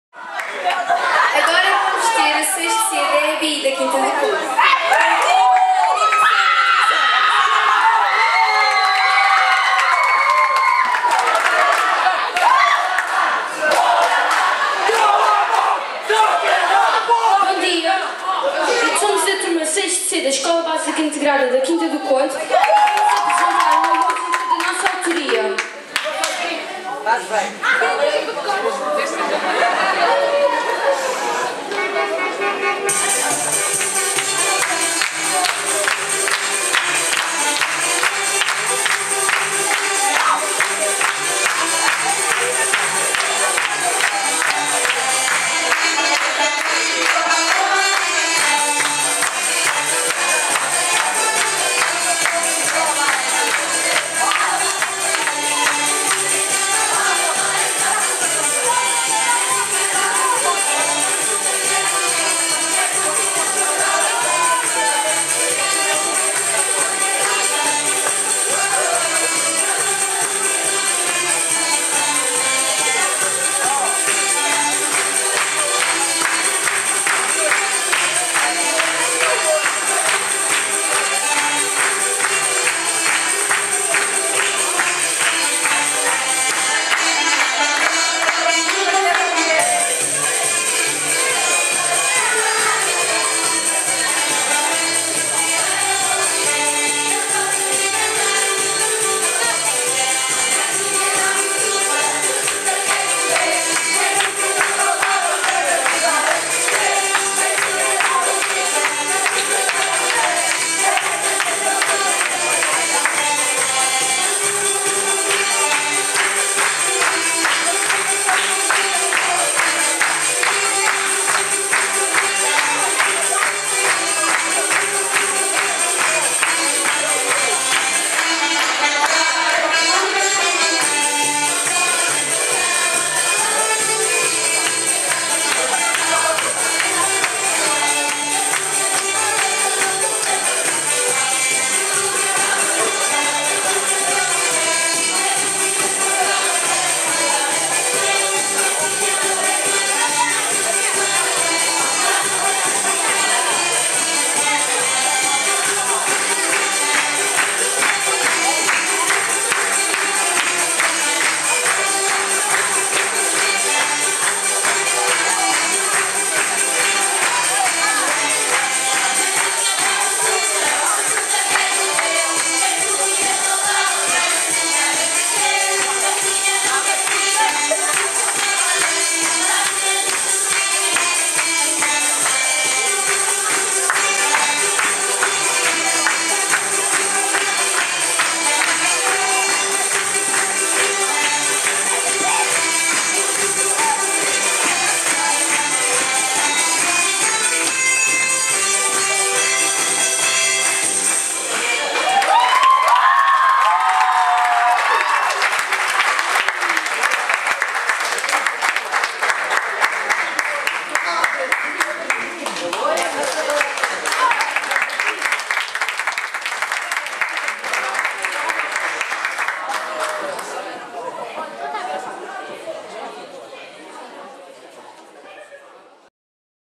GISC 2014 - A Correr e a Saltar - 6º C da Escola Básica Integrada da Quinta do Conde
Imagens animadas da atuação dos alunos do 6ºC da Escola Básica Integrada da Quinta do Conde no  XIV Congresso do Grupo de Intervenção em Saúde Comunitária (GISC).

"Jovens Responsáveis, Adultos Saudáveis foi o tema do XIV congresso do Grupo de Intervenção em Saúde Comunitária (GISC), que contou com a participação de alunos do Jardim de Infância,  2.º, 3.º ciclo e secundário das escolas do concelho de Sesimbra, e que decorreu no dia 23 de maio, a partir das 9.30 horas, no Cineteatro Municipal João Mota, em Sesimbra.

Os Jovens e as Suas Mudanças, Saber Comer para Bem Viver ou Viver e Sentir a Sexualidade foram alguns dos temas abordados durante o congresso.

No final, os jovens puderam assistir à peça de teatro O Que Estás a Fazer?!, dinamizada pela Fundação PT.

Promover o debate de ideias sobre várias realidades relacionadas com a saúde dos jovens e aproximá-los dos serviços públicos é o objetivo do congresso do GISC, promovido pela Câmara Municipal, Agrupamento de Centros de Saúde da Arrábida e agrupamentos de escolas do concelho, com o apoio da GNR -- Escola Segura."

A reportagem foto e vídeo do evento foi efetuada como exercício prático por alunos do Curso Vocacional de Cozinha/Pastelaria e Foto e dos alunos do 1º do Curso CEF Operador de Fotografia do Agrupamento de Escolas da Quinta do Conde.

Publicação on-line no âmbito do Projecto pedagógico AEQC-TV do Agrupamento de Escolas da Quinta do Conde:

Canais:AEQCTV